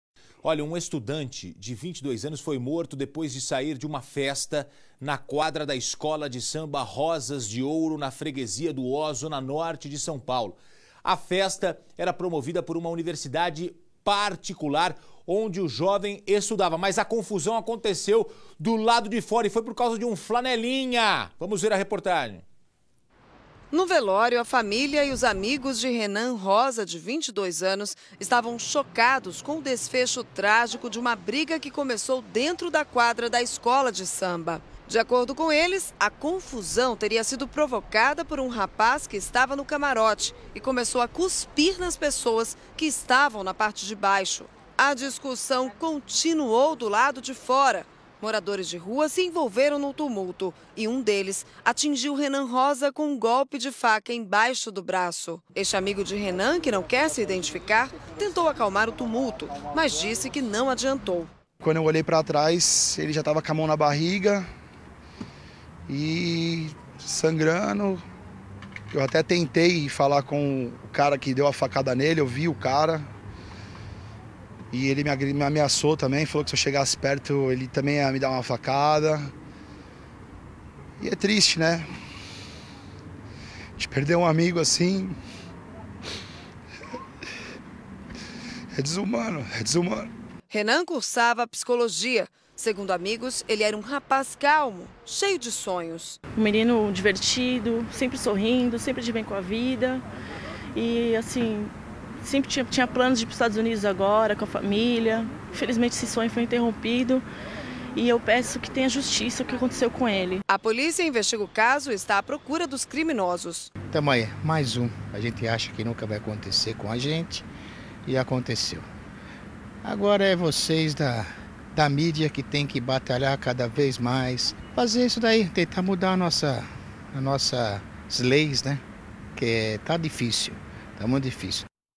Estudante de 22 anos é morto em festa de escola de samba em SP
O universitário de 22 anos foi morto com duas facadas no peito na manhã do sábado (11), na saída de uma festa organizada por alunos do Mackenzie na quadra da escola de samba Rosas de Ouro, no bairro do Limão, zona norte de SP. O jovem teria discutido com um flanelinha, quando um suposto morador de rua apareceu e deu duas facadas na vítima.